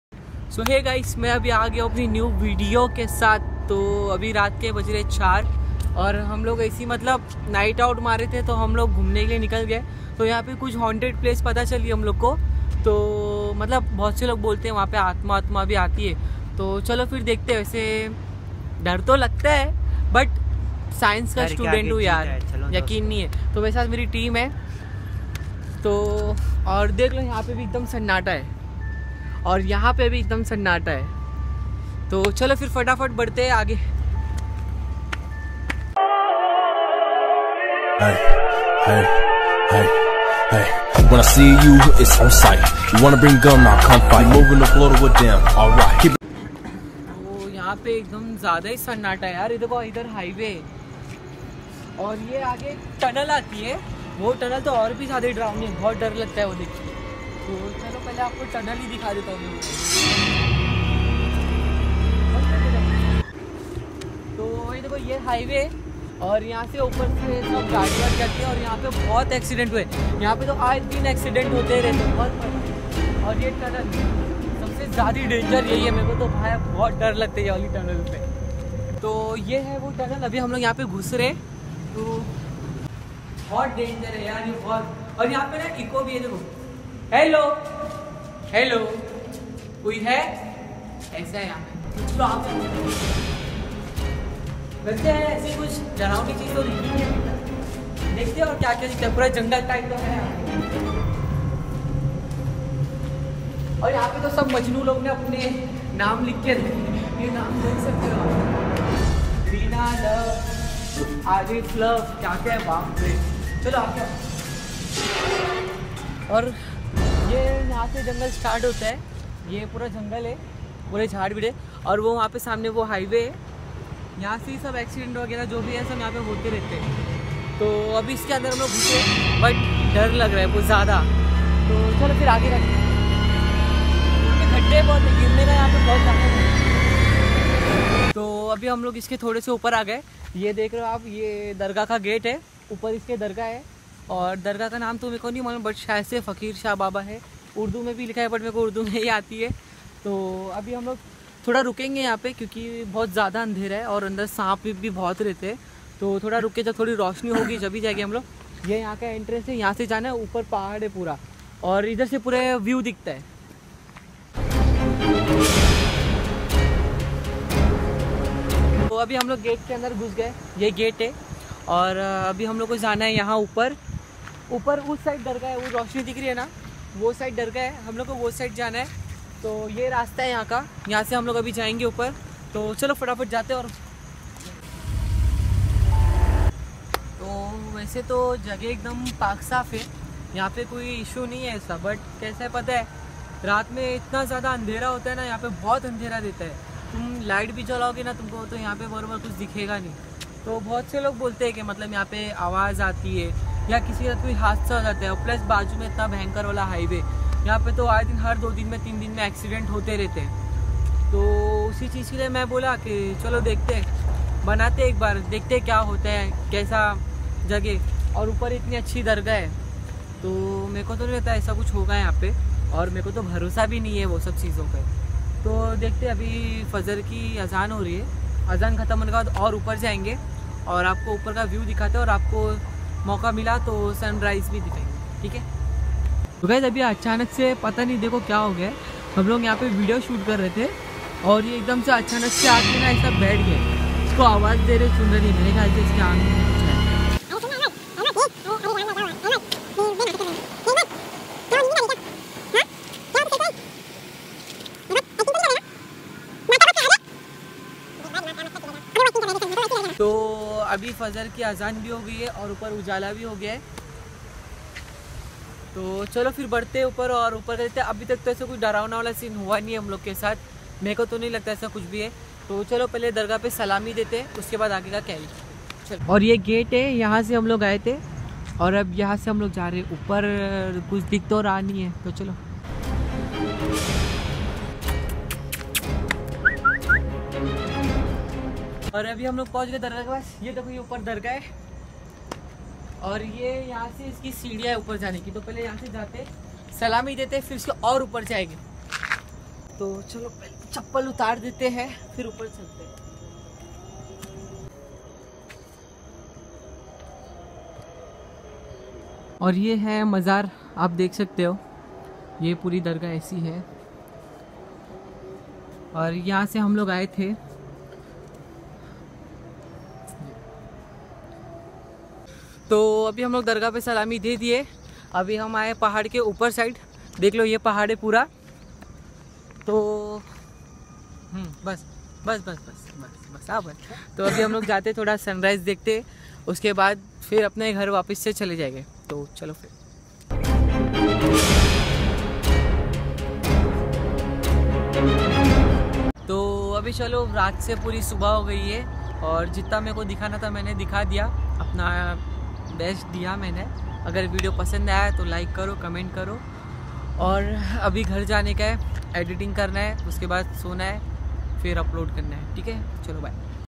Honted vlog 🥶 || exploring mountain || night out
Yeh ek Jhoot baat hai ki waha bhoot hai . Waha bhot pyaari dargah hai

Waise bhi Hun bhoot pa vishwash Nahi karta .

So yeh vlog bass entertainment ke liya hai so isko enjoy karo 😂

Aur kuch log hai joh mera content chori karta hai Aur bolta hai Mai content chori karta hu so please yeh sub faltu harkat Mat karo 🙏🏻

Tysm my team

Ahmed ,Altamash , Ismail

Shoot by 👇🏻👇🏻👇🏻👇🏻

Shoot location : mumbra khadi machine